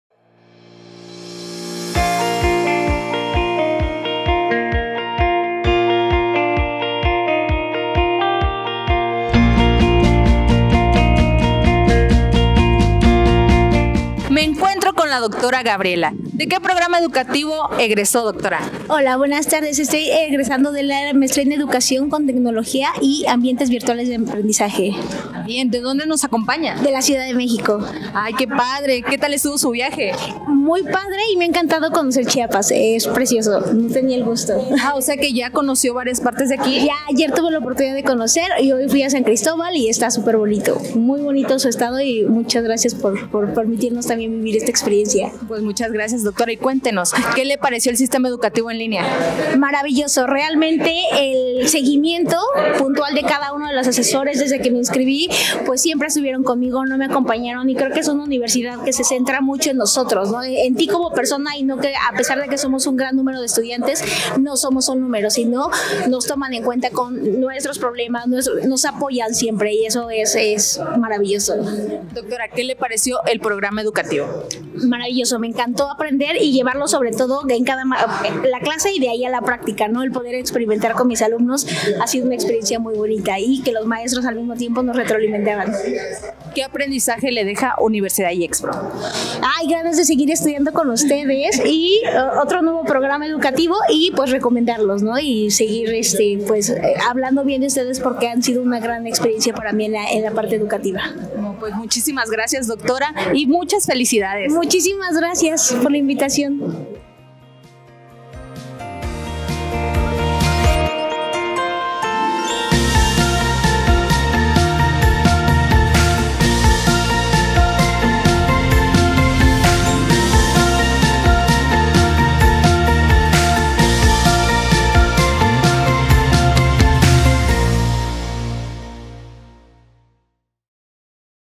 Entrevista a Egresados IEXPRO 2023 - Dra. Ana Gabriela Robles M
Les invitamos a ver la siguiente entrevista a una de nuestras graduadas: la Dra. Ana Gabriela Robles M., egresada del Doctorado en Educación con Tecnologías de Aprendizaje y Conocimiento en Entornos Virtuales, que nos compartió sus experiencias al estudiar en Universidad IEXPRO.